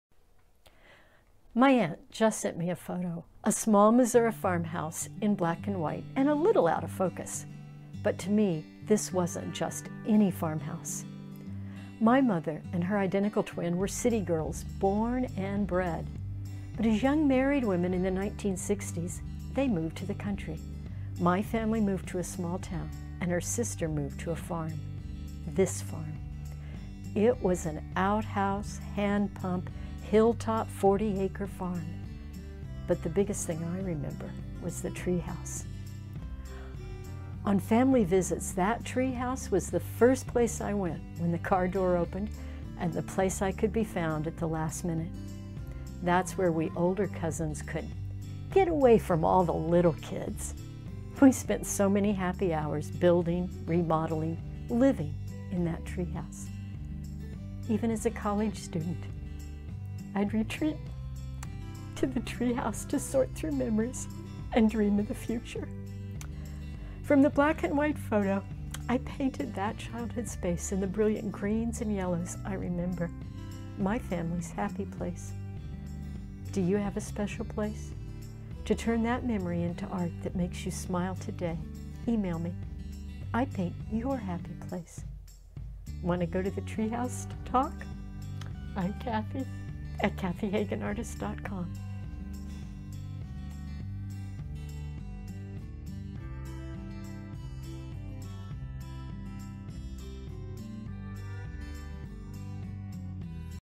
Painting a Tree House Sactuary in Watercolor
I'm Cathy, passionate about transparent watercolor and painting your happy place.

Painting your happy place! I do transparent watercolor because I want to be transparent in every area of my life.